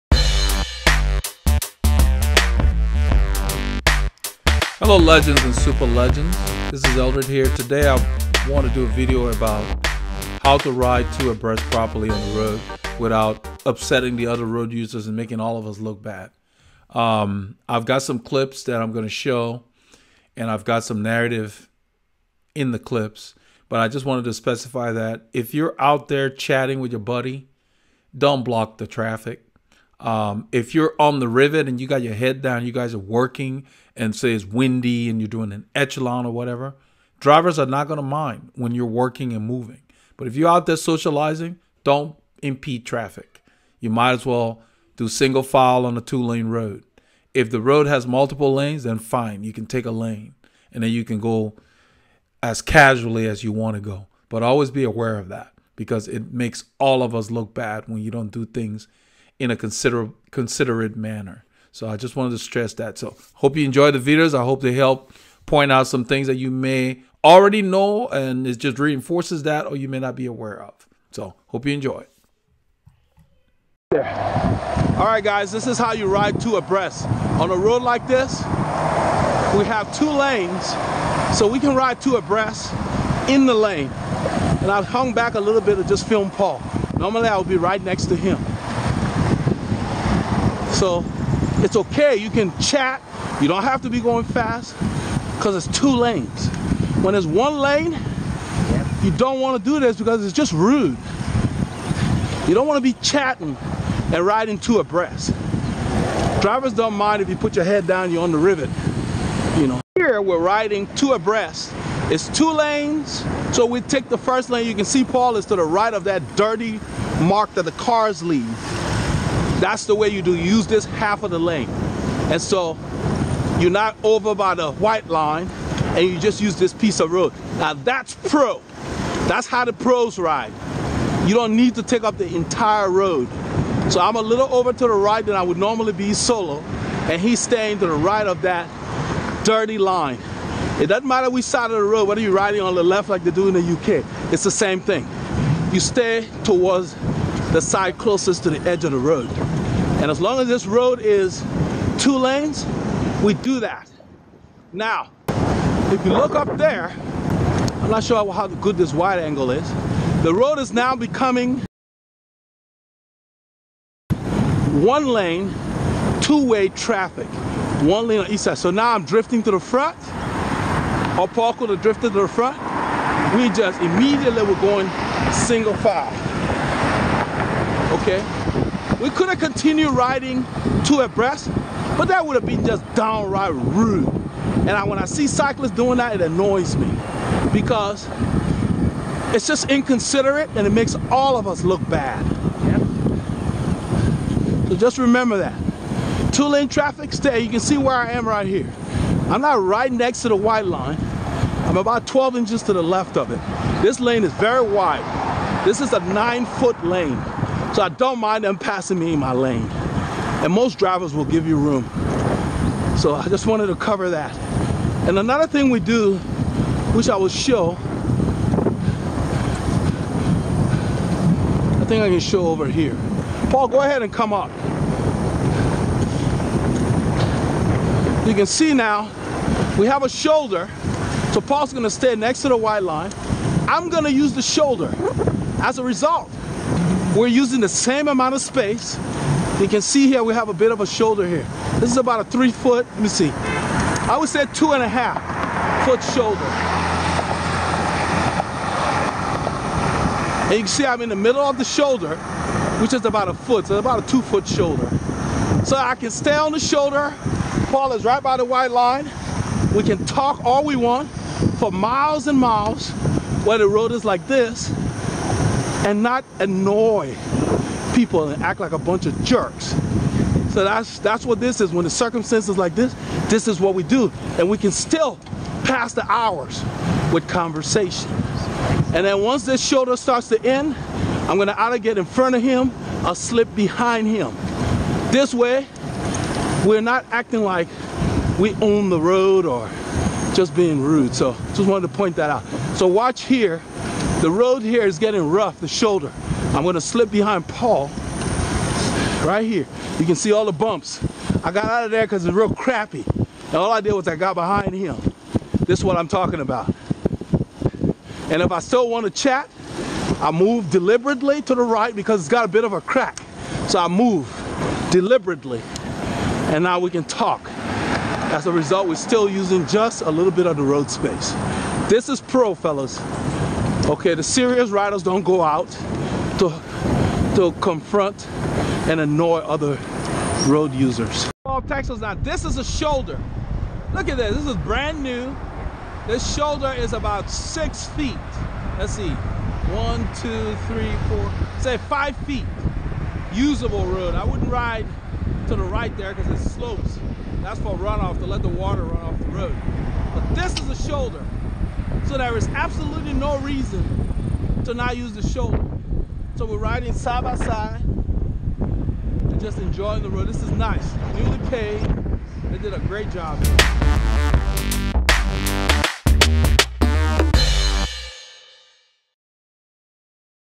When & how to ride two abreast
Learn the proper way to ride 2 abreast on the road
Welcome to Veloharmony, the common sense Channel